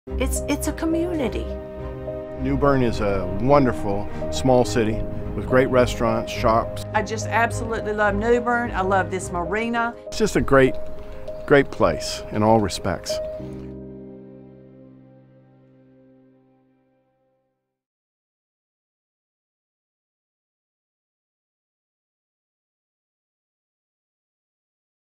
New Bern Grand Marina Boat Slip Promo
Looking for a unique Christmas gift for the special boater in your life?  New Bern Grand Marina Yacht Club offers a slips and so much more in historic New Bern, NC.  If you're traveling the ICW or looking for a place to settle down New Bern offers the best choice in North Carolina.

For over 30 years, the New Bern Grand Marina Yacht Club has been referred to as the “Jewel of historic New Bern”. Pictured and featured in many local, state, regional, and national publications for its beauty. The New Bern Grand offers a unique story as it was masterfully designed to entice visitors and tourists to visit New Bern. Situated at the convergence of the Neuse &amp; Trent Rivers, 21 nautical miles off the Intracoastal Waterway it was developed and planned to offer the best year round deep water marina on the east coast and boasts a 700 foot wave attenuator and floating docks.  Nothing was spared in terms of expense. It’s not uncommon to walk by the marina and  be mesmerized by its placid protected waters.

This gift would be worth of the Hammacher Schlemmer catalog.  Check out the video and website to learn more or just call!